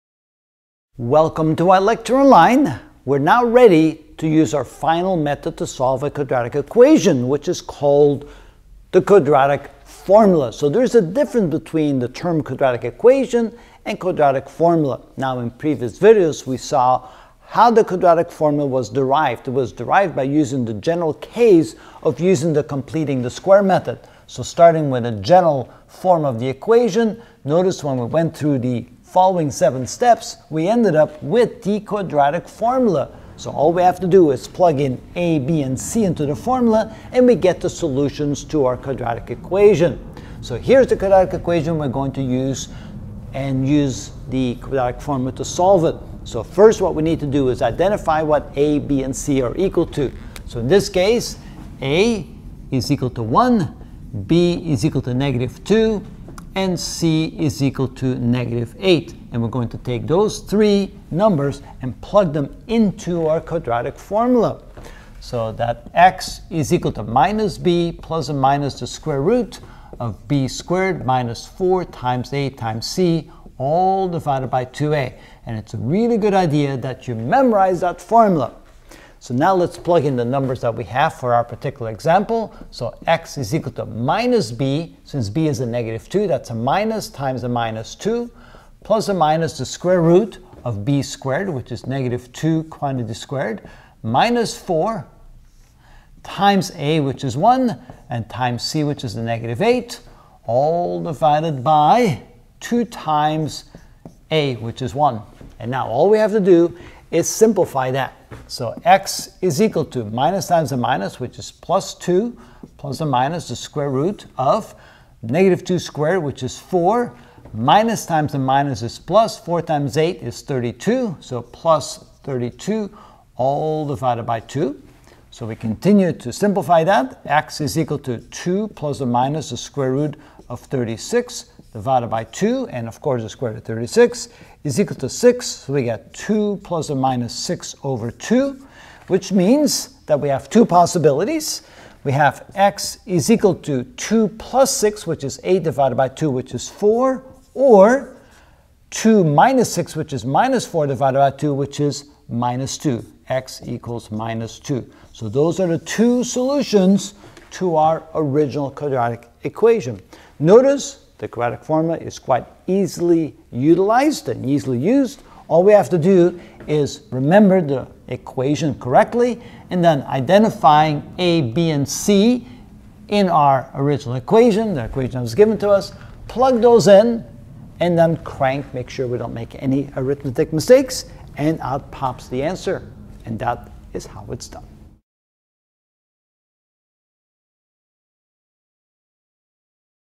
Algebra - Ch. 25: Solving a Quadratic Equation (20 of 25) Solve by Quadratic Equation Ex. 1
We will find x=? or x=? given x^2-2x-8=0, a quadratic equation, using the Quadratic Formula. Example 1